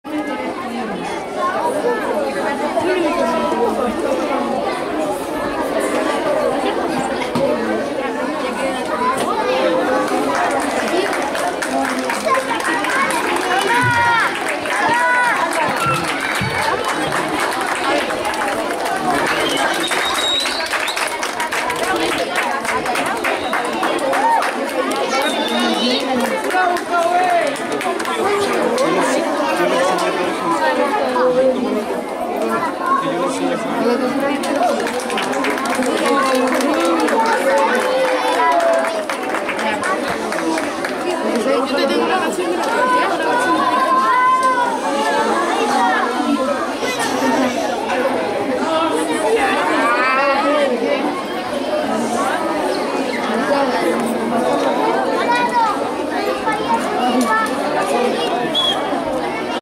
Entrega de Reconocimiento Apoderados Segundo Ciclo - Día del Apoderado 2018
Realizado en la Escuela General Sofanor Parra de la ciudad de San Carlos.